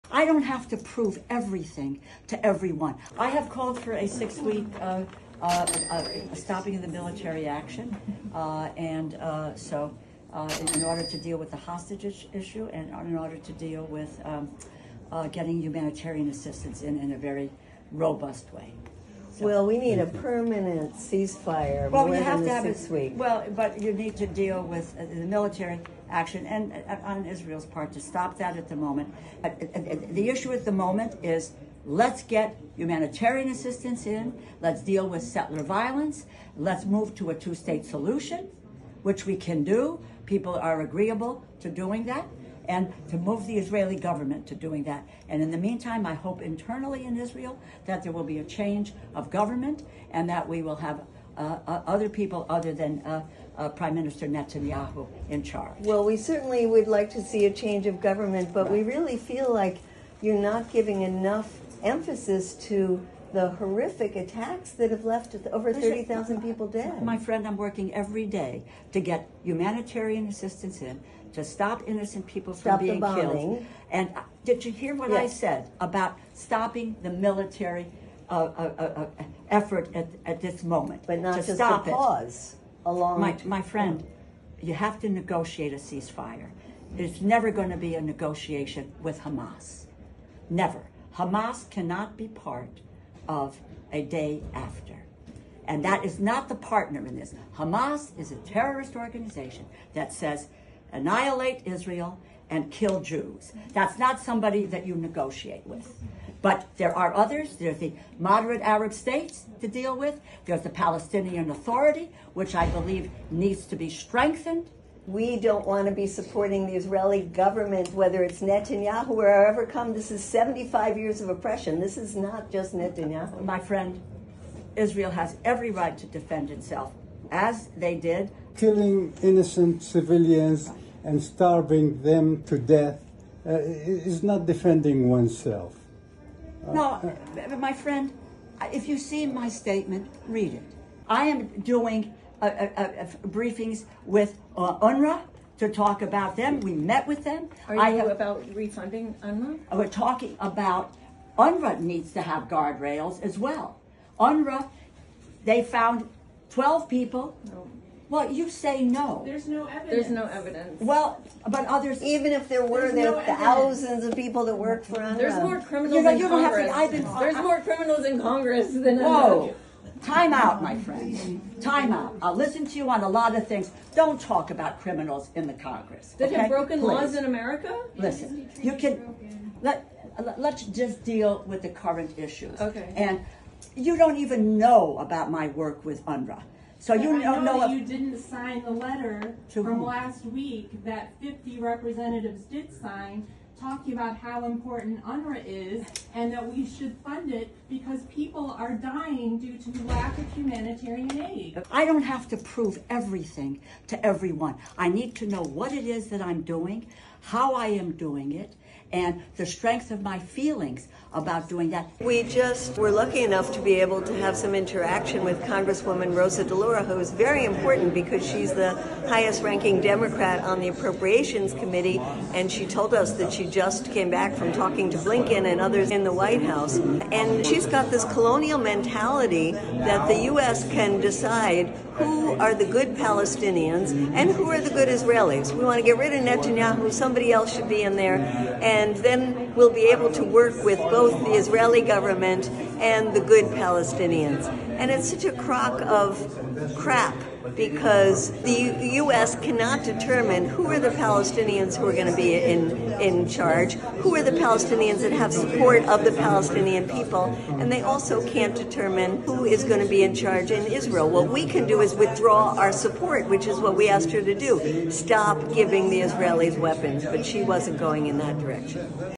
Why won't Rep. DeLauro Stop Arming Israel?
DeLauro says she wants to stop civilian deaths, but won’t call for a permanent ceasefire and won’t oppose sending weapons to Israel.

She says she supports getting humanitarian assistance in, but she hasn’t called to restore funding to UNRWA.

Worst of all, she has the colonial mindset that the U.S. should dictate who governs and represents Palestinians.

Even though the majority of Americans want to stop sending weapons to Israel, DeLauro won’t. She’s taken over half a million dollars from the arms industry and pro-Israel lobby.

Feminists stand against genocide and occupation in solidarity with women everywhere. Rep. DeLauro doesn’t.